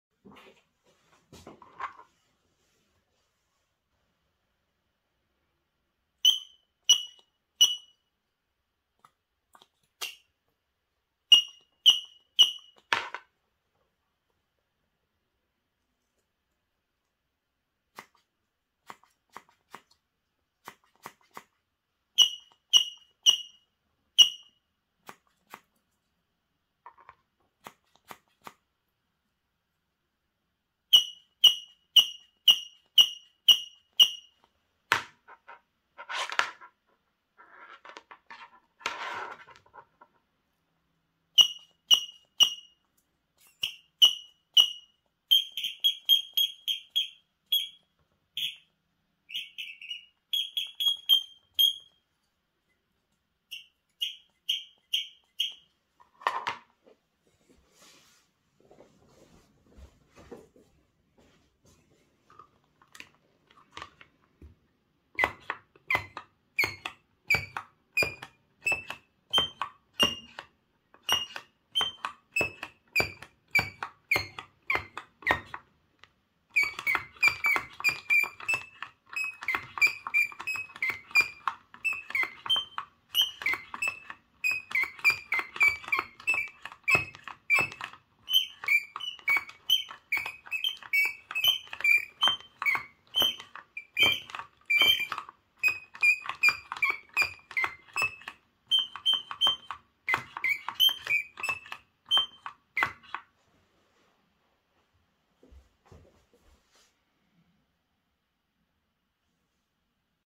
LEGO PneumaticOrgan
The old LEGO pneumatic distribution blocks make a high pitch sound when air passes through their internal valve.